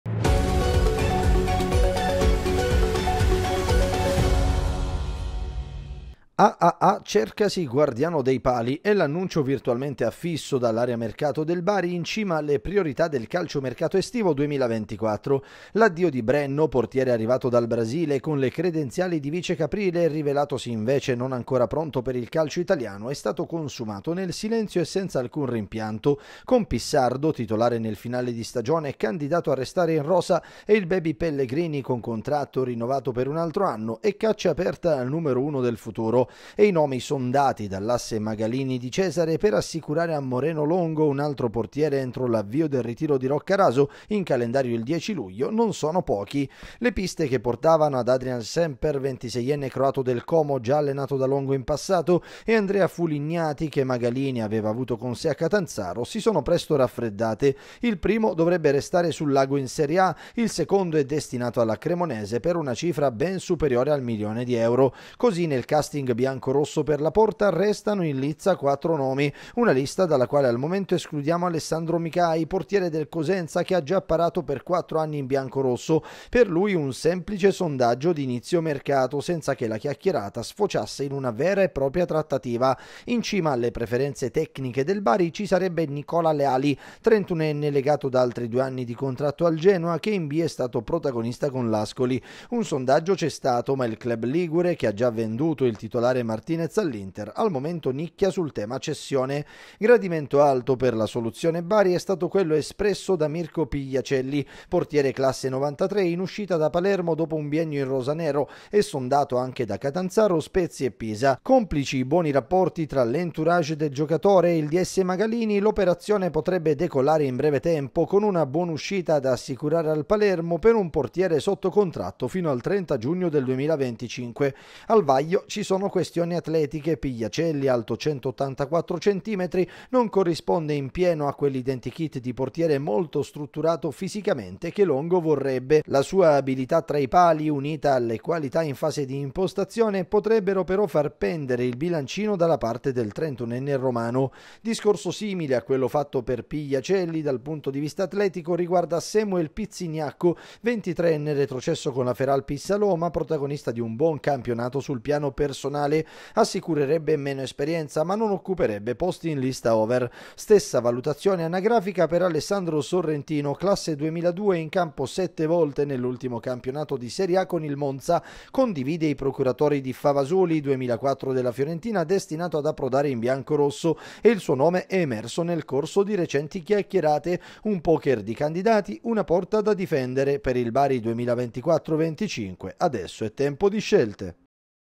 BARI | Bari al lavoro per un nuovo portiere: in lizza Leali, Pizzignacco e Pigliacelli
Magalini e Di Cesare a Milano